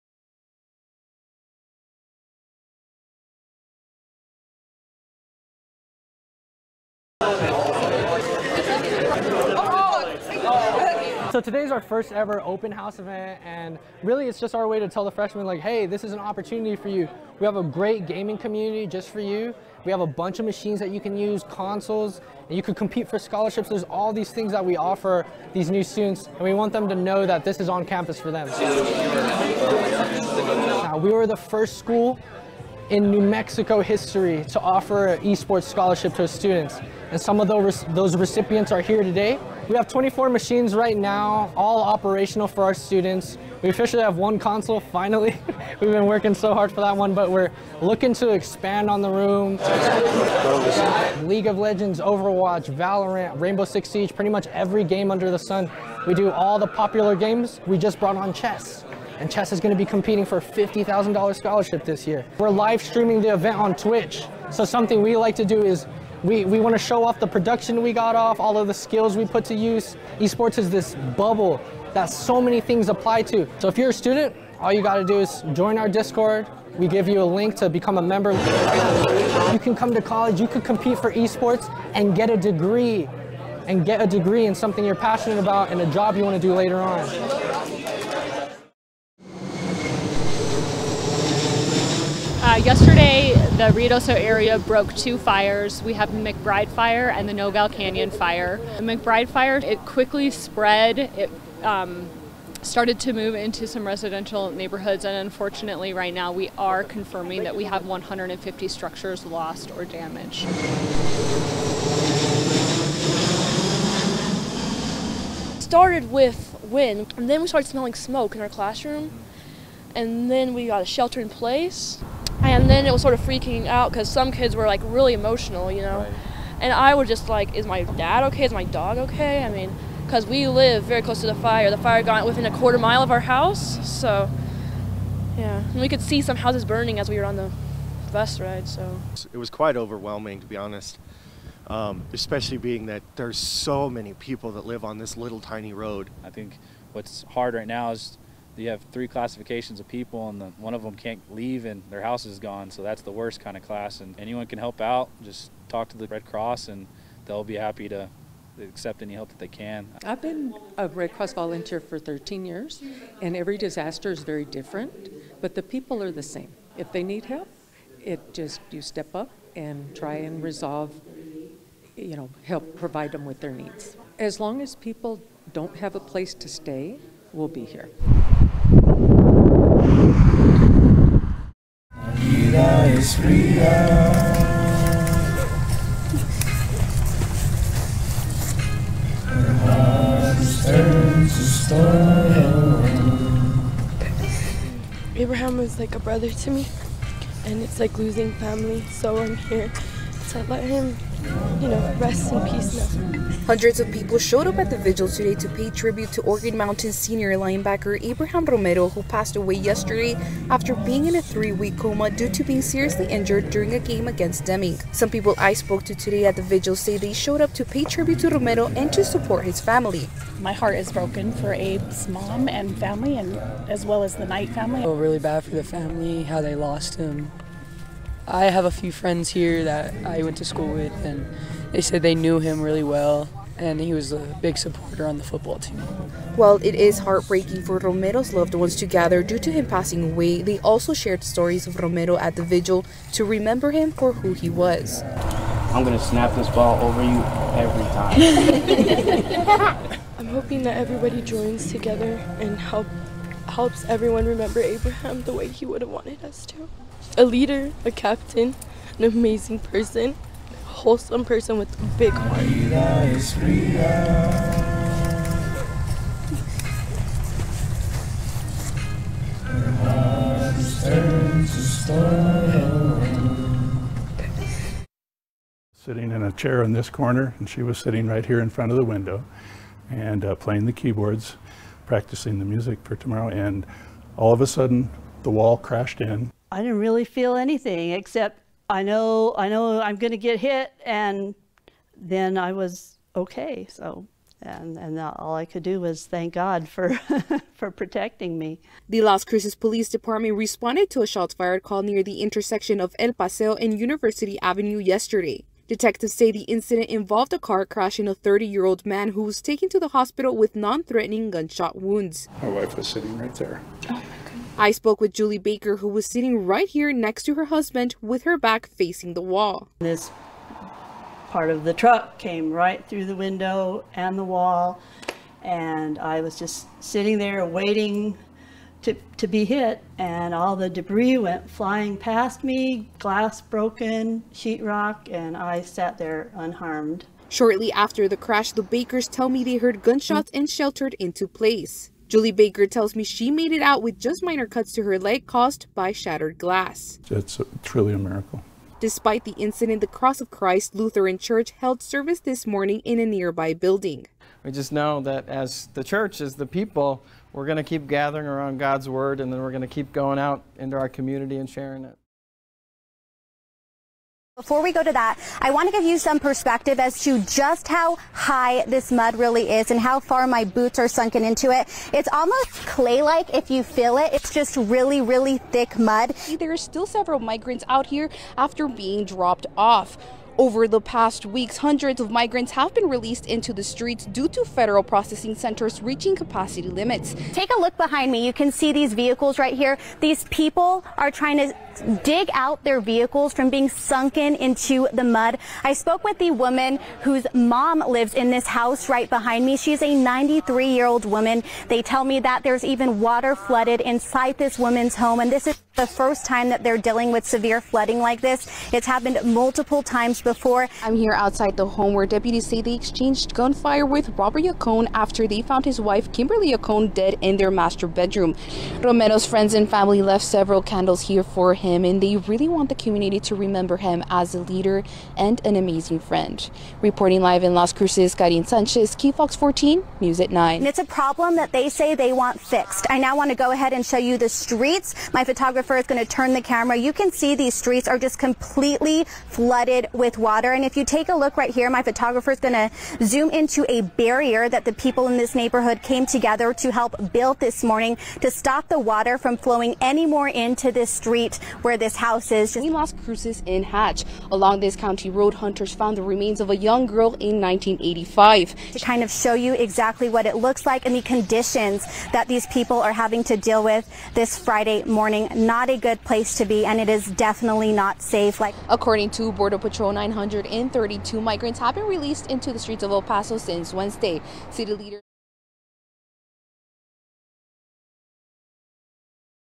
Sergio Delgado Reel 2022
Photojournalist reel for Sergio Delgado. Currently covering El Paso TX, Las Cruces NM and the U.S. Mexico Border.